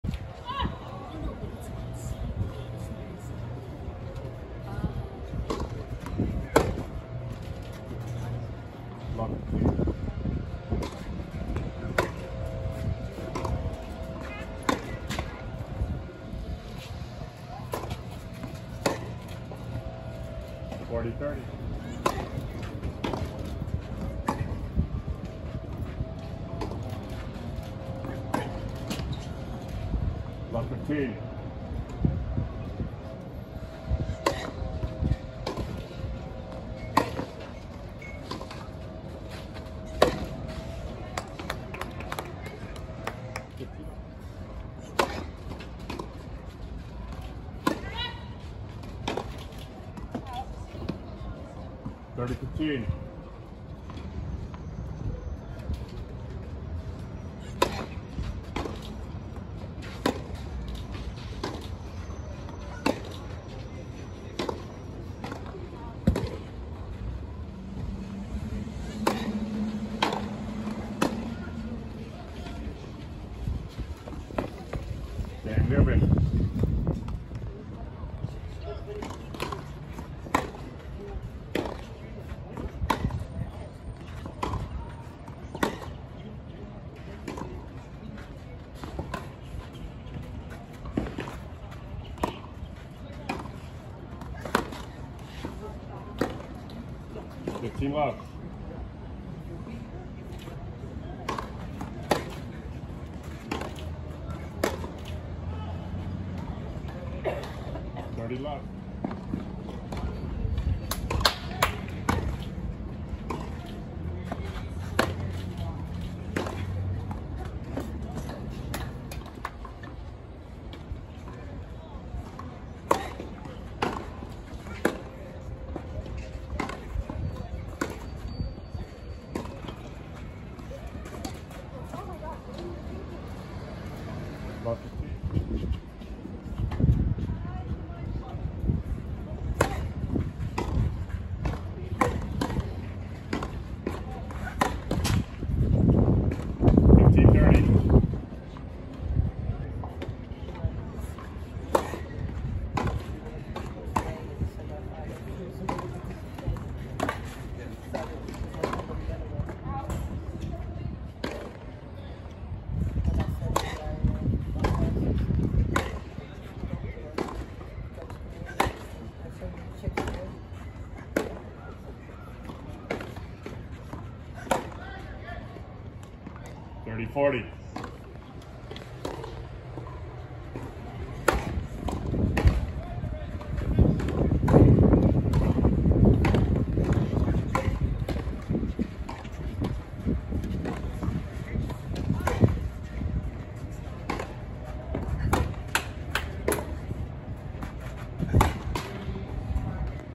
Newman Jr Orange Bowl G14s Final 2023
Welles Newman was defeated by Jana Kovackova 6-3, 7-6(5) in Junior Orange Bowl G14s final, December 21, 2023, Salvadore Park, Coral Gables Florida